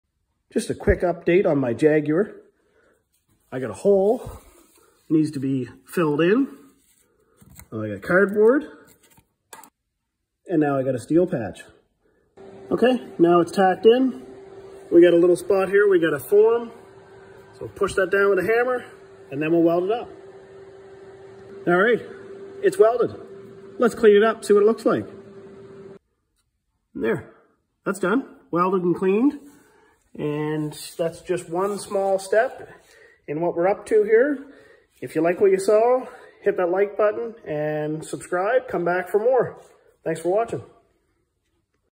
Custom metal work on my Jaguar!
Total reconstruction of a vintage Jaguar!
This will be a one off custom, wide body with a antique race car vibe.
I call it the DNC8